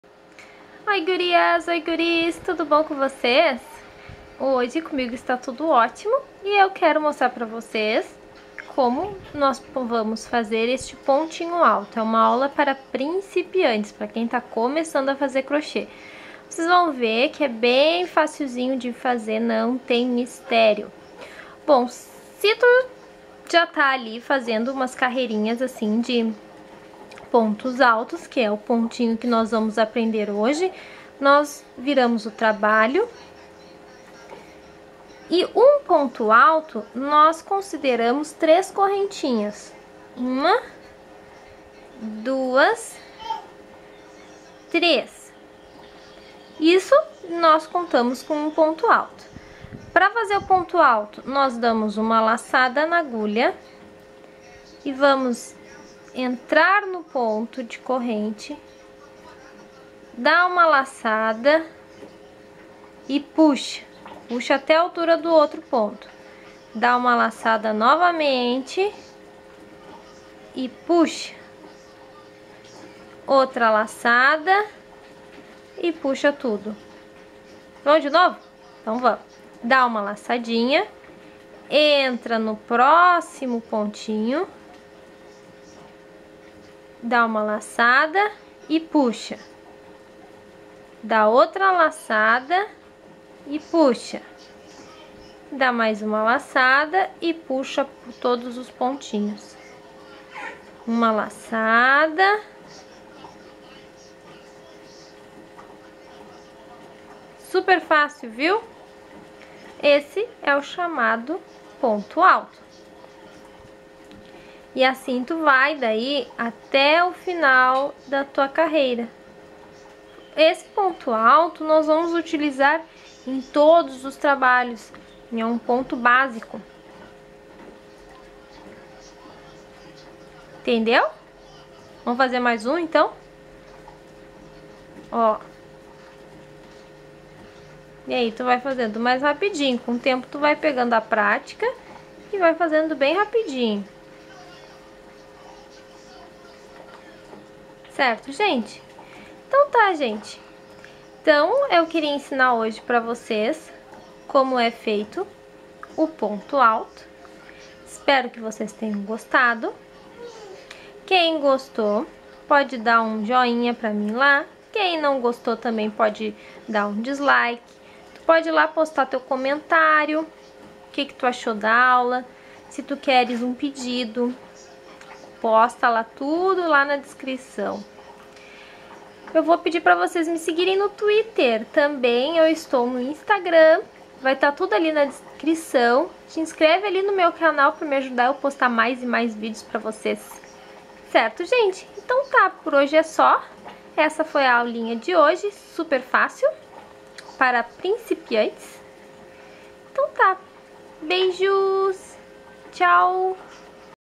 Vídeo Aula: Ponto Alto de Crochê
Oi pessoal!!!

Hoje eu vou ensinar para vocês o ponto alto de crochê, é uma aula para principiantes.
Compartilhem!!! Curtam!!!
Sigam-me no twitter @GabiArtesanatos

Beijoosss!!!